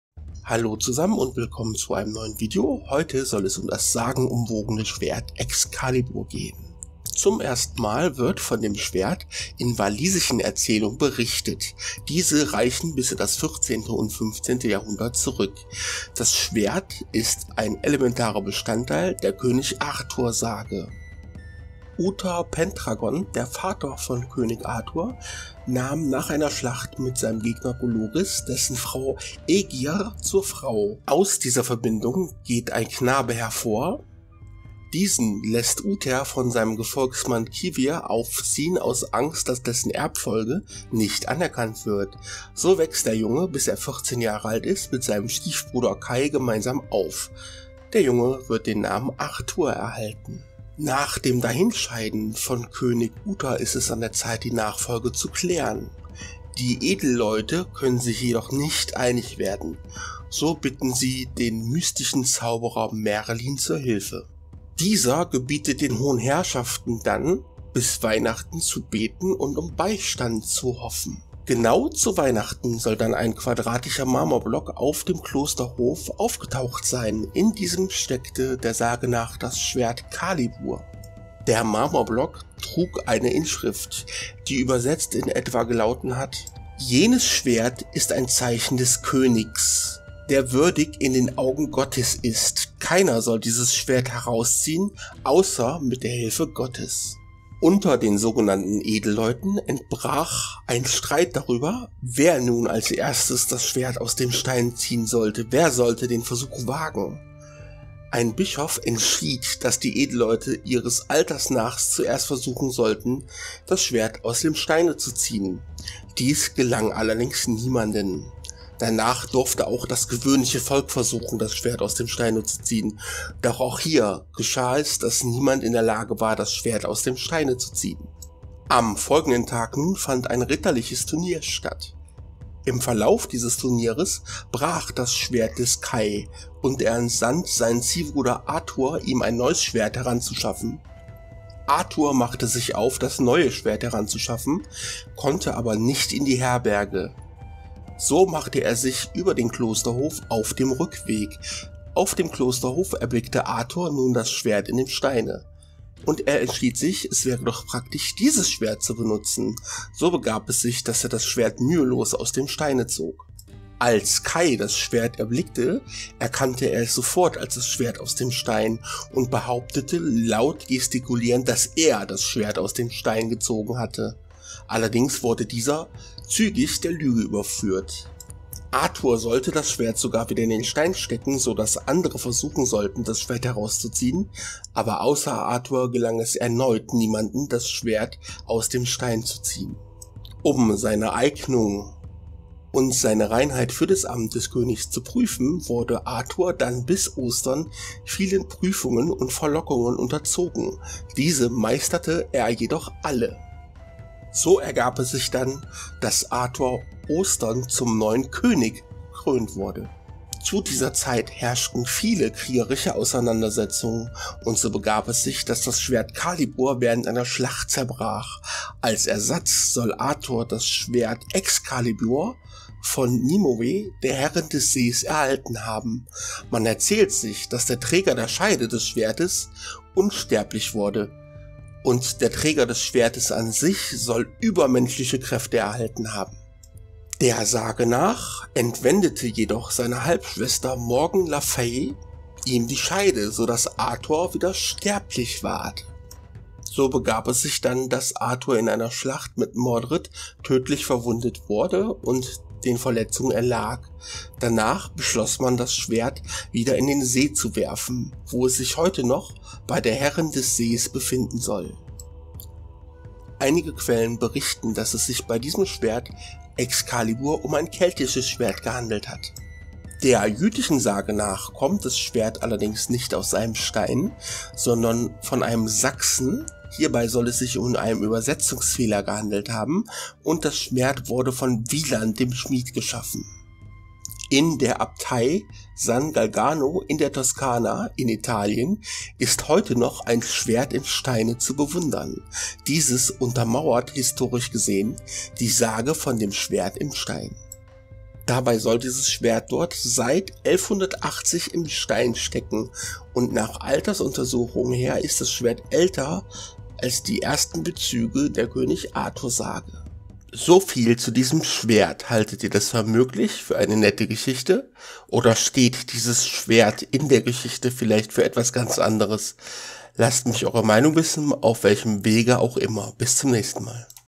Das Schwert Excalibur und seine Geschichte - Mfiles 002
Hier geht es um das Sagen umwogene Schwert Excalibur  bzw Calibur.
und die König Artihus Sage wird kurz angeschnitten.

Eure Kritik ist gerne gesehen:)